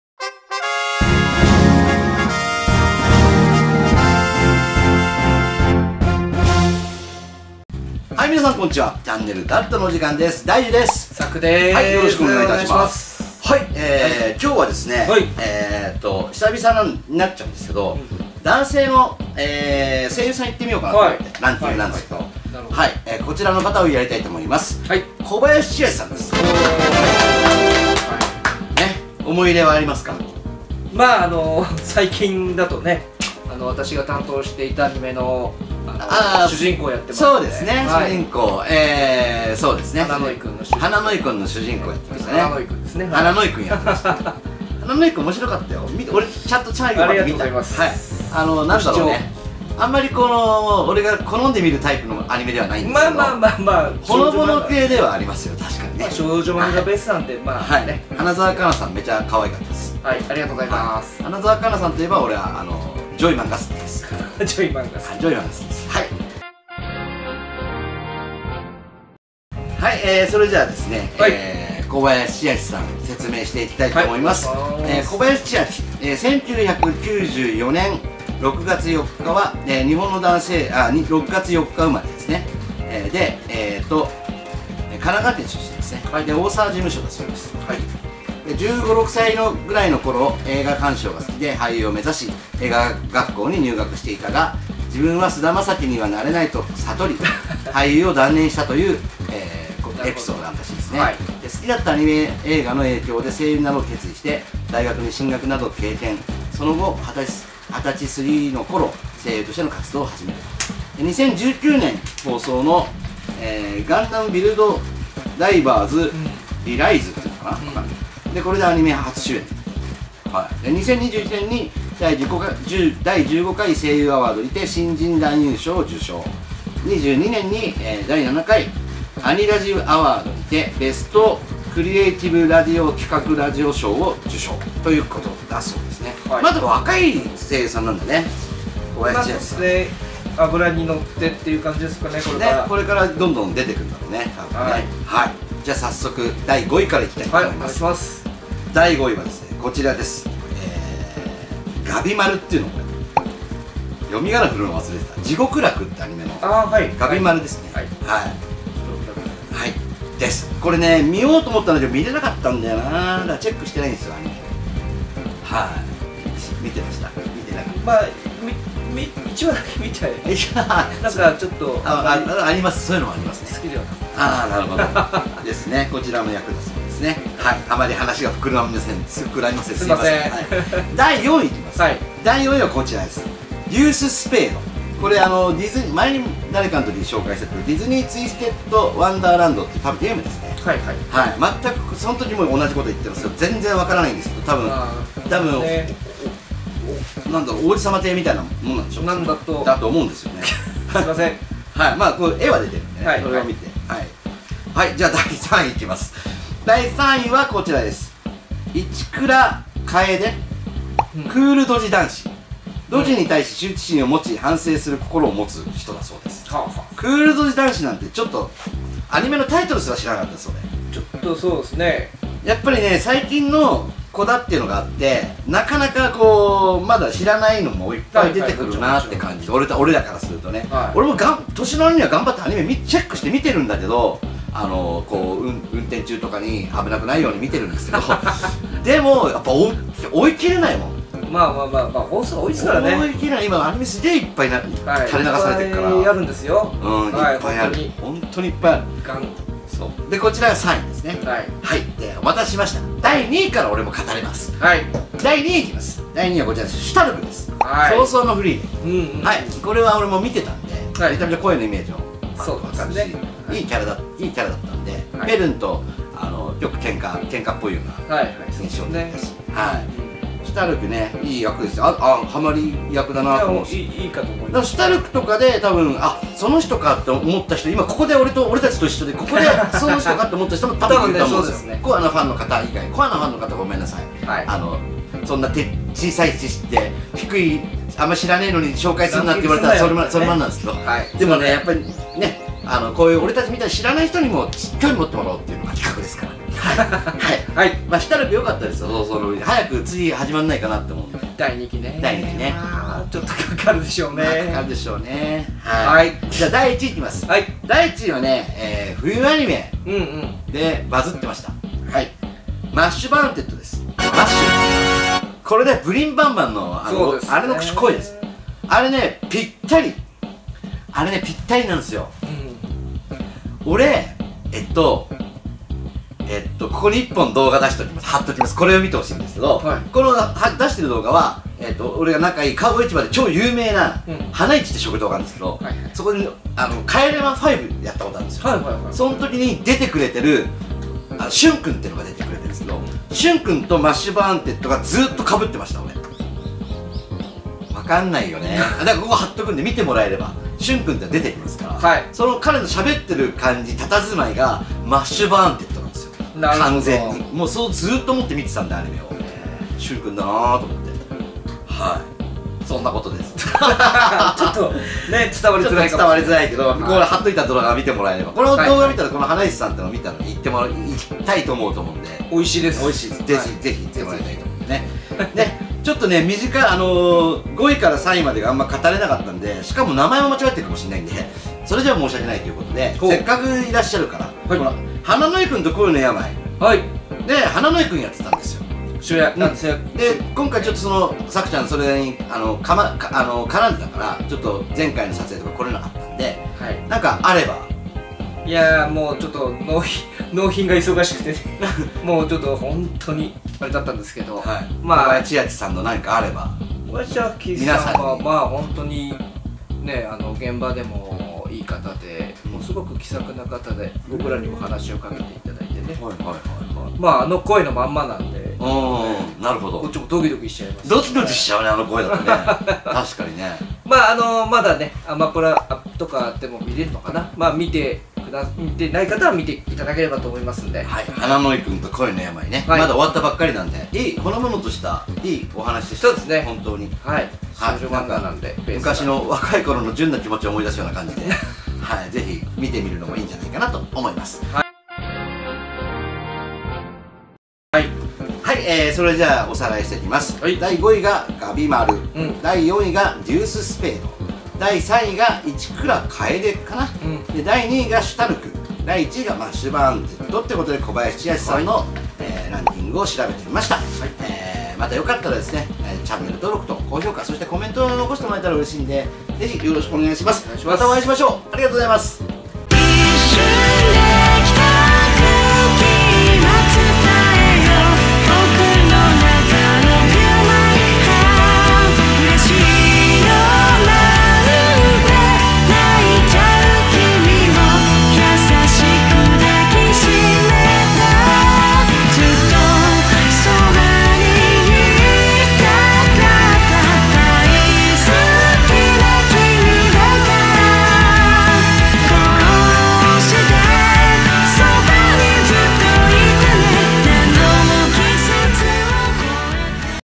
【人気ランキング】声優・小林千晃の好きなキャラTOP５
今回は声優・小林千晃が演じたの人気キャラクターランキングをお届けします。
ぜひ皆様の好きな小林千晃さんのキャラクターがあったらコメント欄にお願いいたします。
最後までご視聴くださいませ。